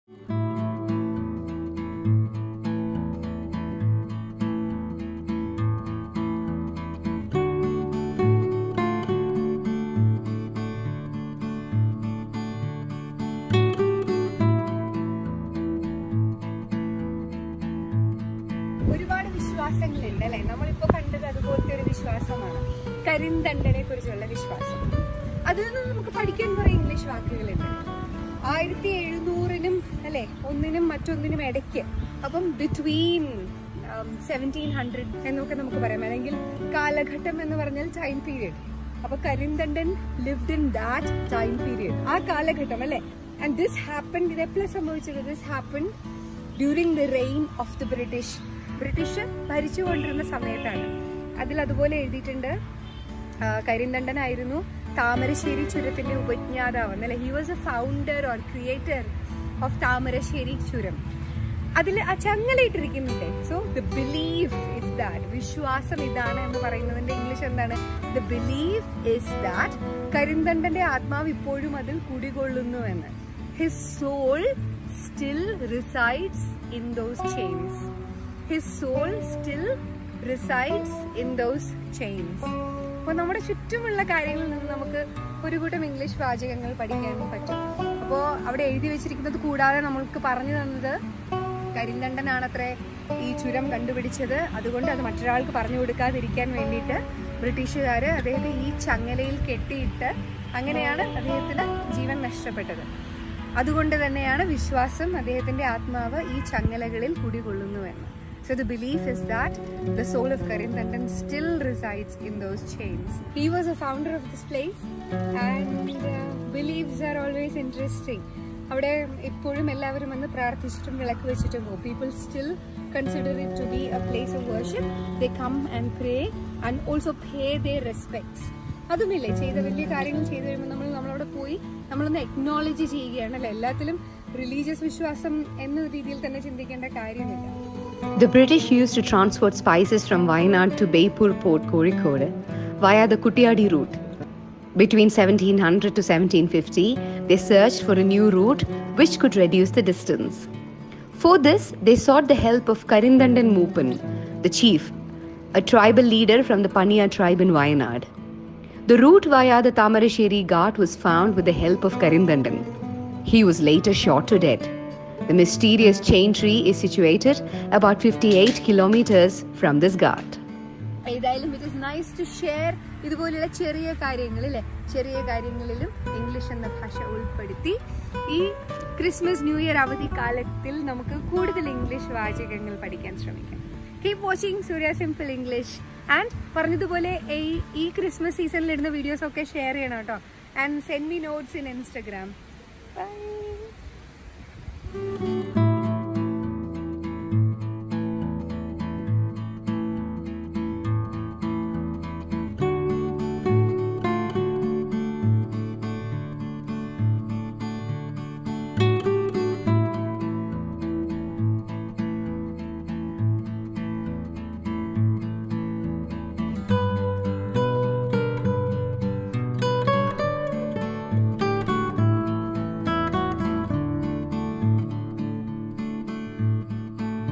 Karinthandan Monument │Learn English words from Karinthandan Churam │കരിന്തണ്ടൻ ചുരം
@SuryasSimpleEnglish കരിന്തണ്ടന്റെ ആത്മാവ് കുടികൊള്ളുന്ന ചങ്ങല Karinthandan Monument - Learn English words from Karinthandan Churam കരിന്തണ്ടൻ ചുരം
The British used to transport spices from Wayanad to BEYPORE Port, Kozhikode via the KUTTIADI route. Between 1700 & 1750, they searched for a new route which could reduce the distance. For which, they sought the help of Karinthandan Moopan (chief), a tribal leader from the Paniya tribe(an ethnic group of India who primarily inhabit Kerala,and the Wayanad, Kozhikode, Kannur and Malappuram districts) .The route via the Thamarassery ghat was found with the help of Karinthandan. He was later shot to death. The mysterious(നിഗൂഢാത്മകമായ) chain tree(ചങ്ങല മരം) is situated about 58 km from this ghat.